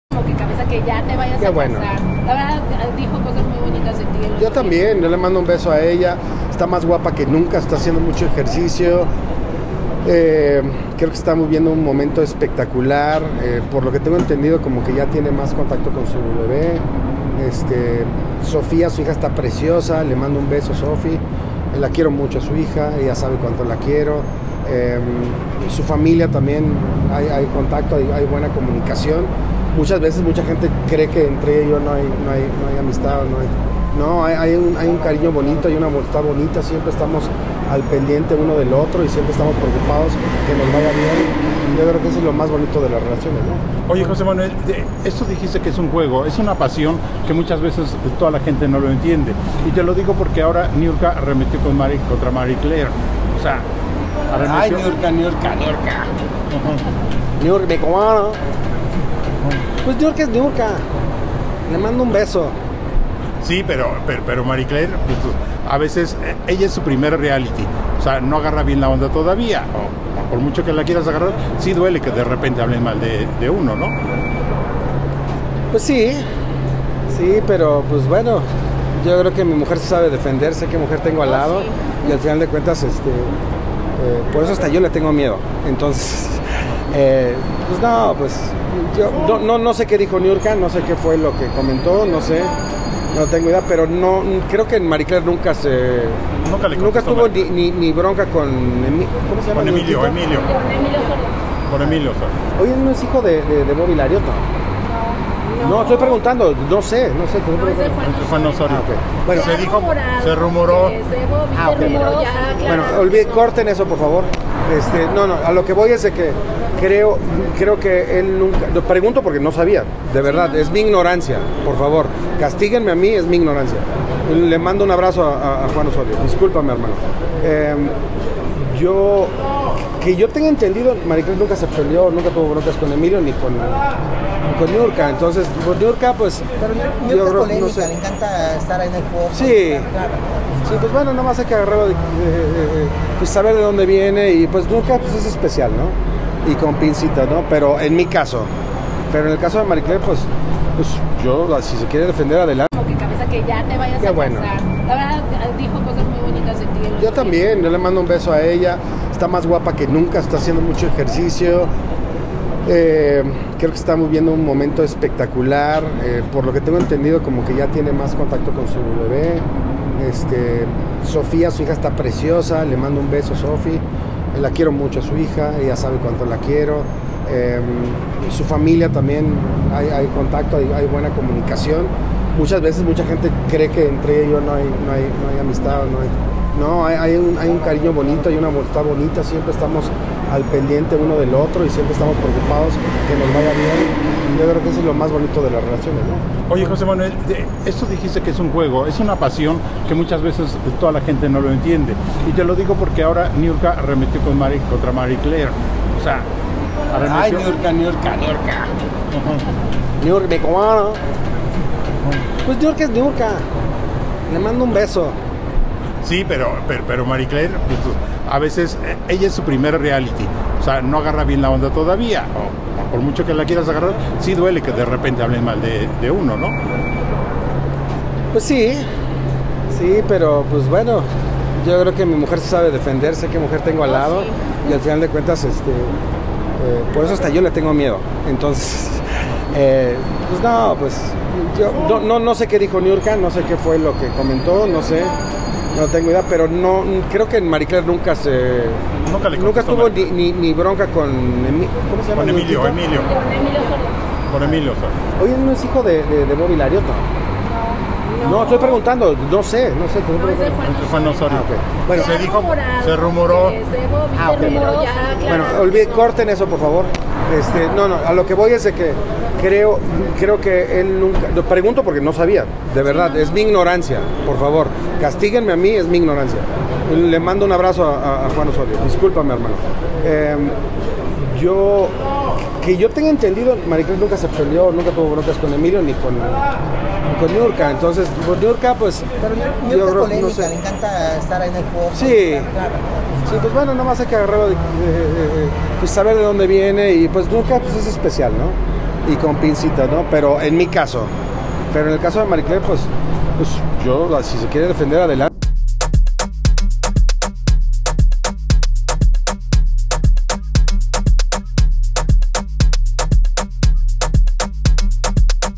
José Manuel Figueroa coquetea con Ninel Conde a pesar de que se va a casar | MICHISMESITO
JoséManuelFigueroa coquetea con NinelConde a pesar de que se va a casar